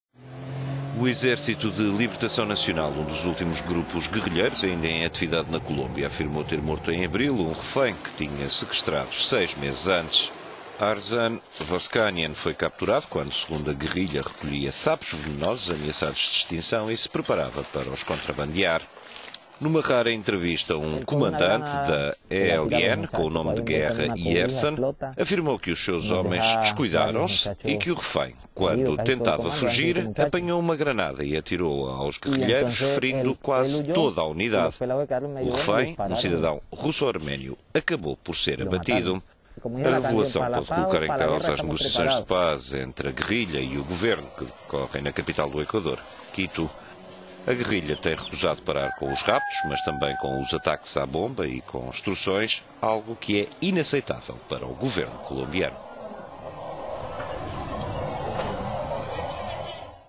Guerrilha colombiana ELN matou refém russo-arménio
O Exército de Libertação Nacional (ELN), um dos últimos grupos guerrilheiros ainda em atividade na Colômbia, afirmou ter morto, em abril, um refém que tinha sequestrado seis meses antes. Arsen Voskanyan foi capturado quando, segundo a guerrilha, recolhia sapos venenosos ameaçados de extinção e se preparava para os contrabandear.

Numa rara entrevista, um comandante da ELN, com o nome de guerra "Yerson", afirmou que os seus "homens descuidaram-se" e que o refém, quando tentava fugir, "apanhou um…